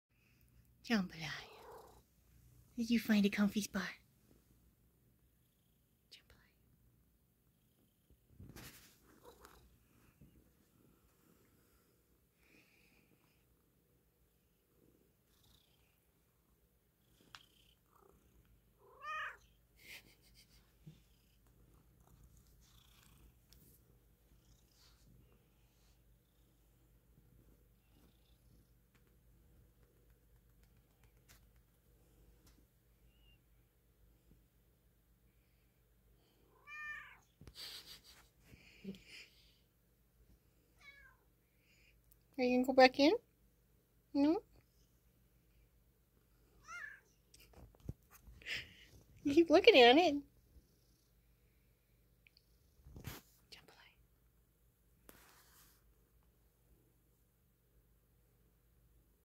This pillow cave is great, hooman!
Jambalaya being comfy in some pillows and blankets

All proceeds go directly to cat related things!